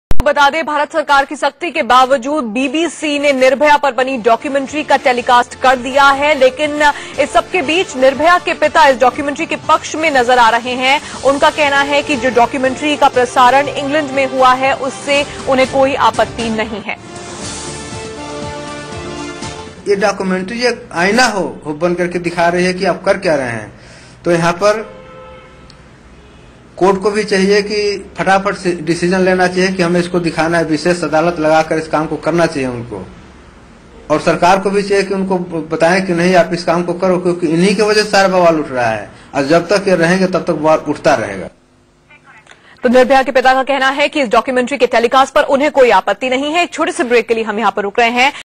Government bans documentary on December 16 gangrape, BBC airs documentary in UK
Even as the government was pressing it not to show the controversial documentary on the December 16 gangrape, BBC decided to go ahead with its telecast, saying that the film had handled the issue "responsibly".

Originally, BBC had decided to show it on March 8, coinciding with International Women's Day. It, however, suddenly decided to advance it following the outrage in India, saying it will enable viewers to see this "incredibly powerful documentary at the earliest opportunity".